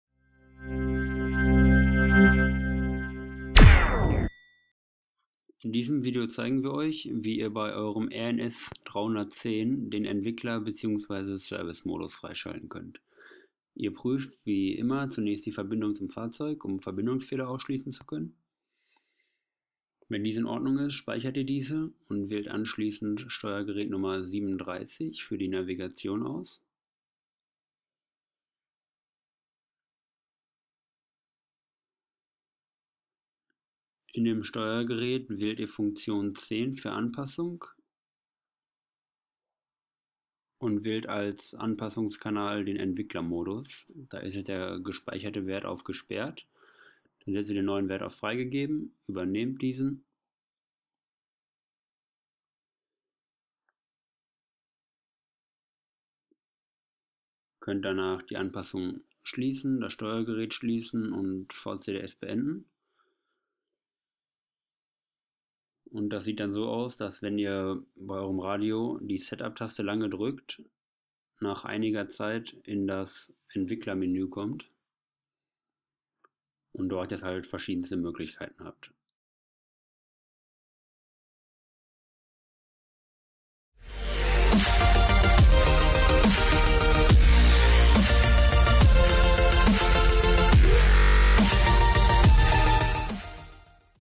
[RNS 310] Entwicklermodus/Servicemodus freischalten mit VCDS Tutorial (HD)
Alle Codierungsarbeiten können mit diesen Sets durchgeführt werden:
STARTER VERSION
PRO VERSION
ULTIMATE VERSION
VCDS hier kaufen
In diesem Video zeigen wir euch, wie ihr bei eurem RNS 310 den Entwicklermodus/Servicemodus für weitere Funktionen freischalten könnt.

Fragen aus den Kommentaren lesen und beantworten wir selbstverständlich.